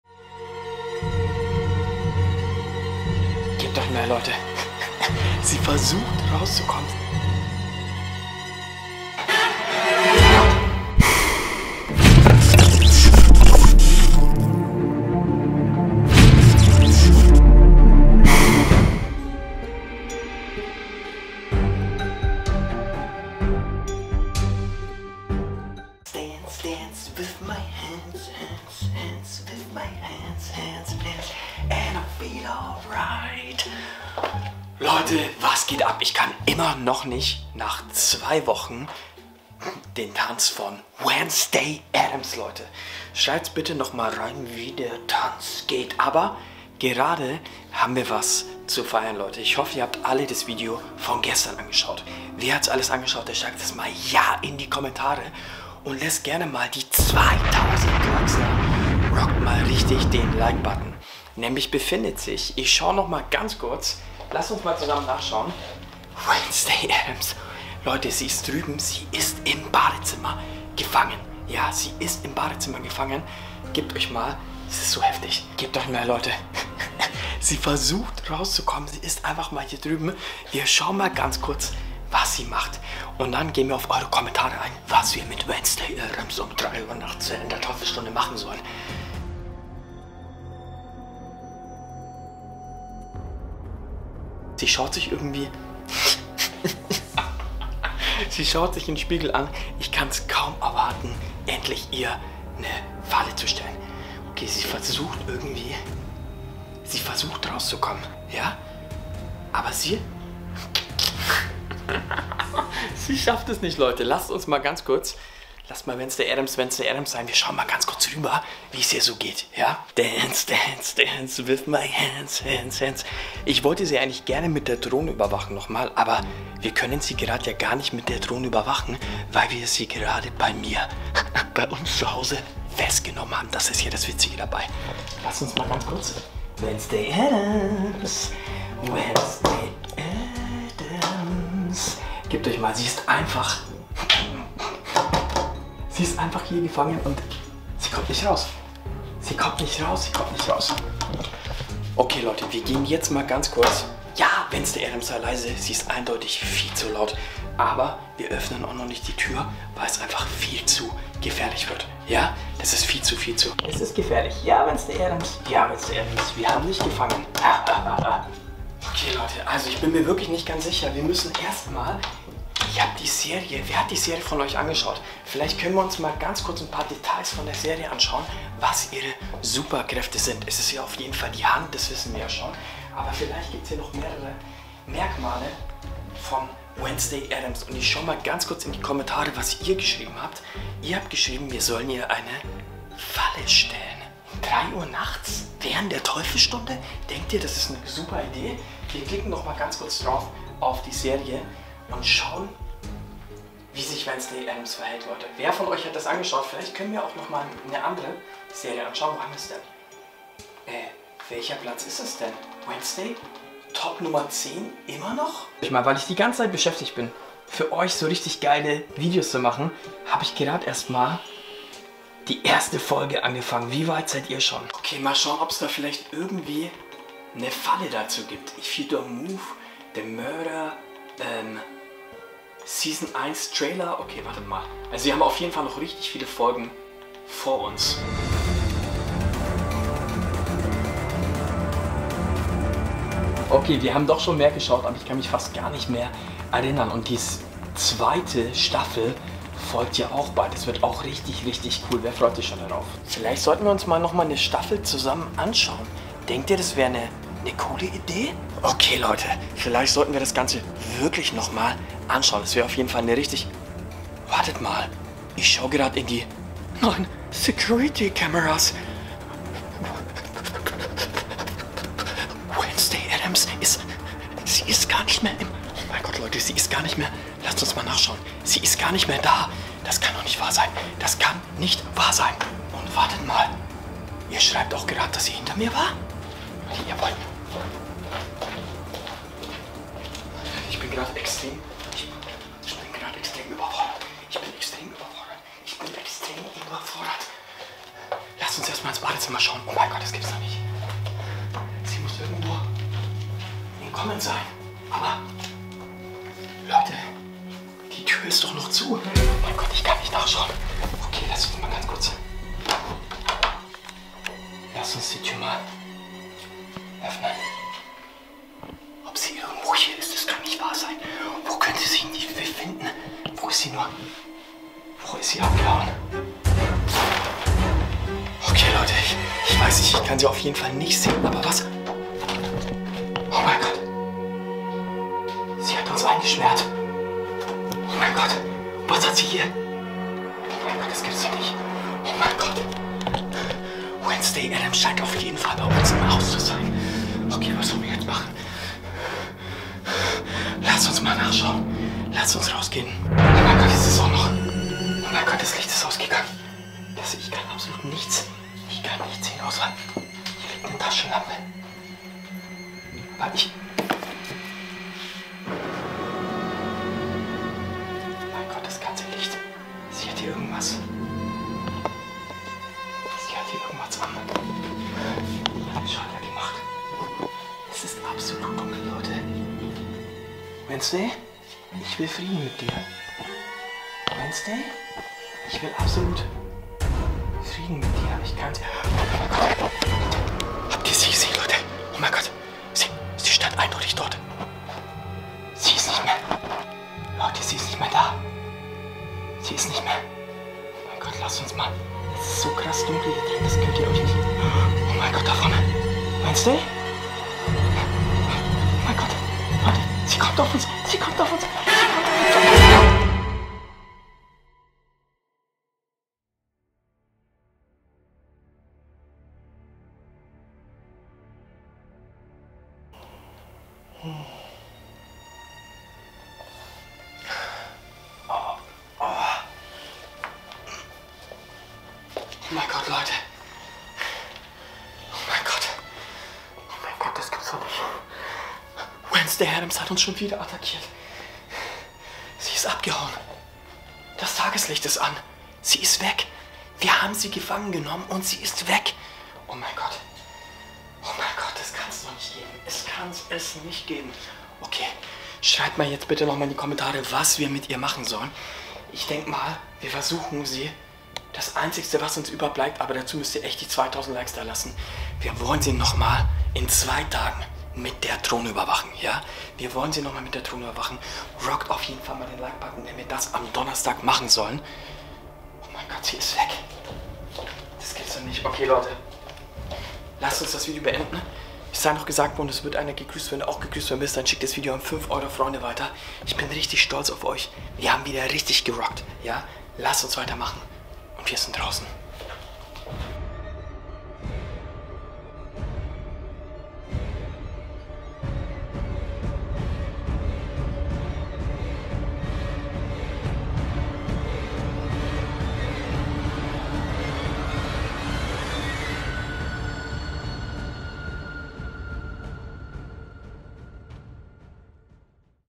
Kamera überwacht WEDNESDAY ADDAMS um 3 UHR nachts, Sie ENTKOMMT!!!! | KAMBERG TV
Wednesday Addams song aus der Netflix Serie kennt jeder. Wir schauen uns Wednesday Addams Deutsch um 3 Uhr nachts an. Sollen wir den Wednesday Addams Tanz auf tiktok nachmachen? Die Hand aus Wednesday Addams! Morgen überwachen wir Wednesday Addams wieder mit der Drohe!

kambergtv & kamberg_tv

Musik und Sounds von Soundstripe

Die Handlungen, Figuren und Geschichten sind reine Fiktion und dienen ausschließlich der Unterhaltung.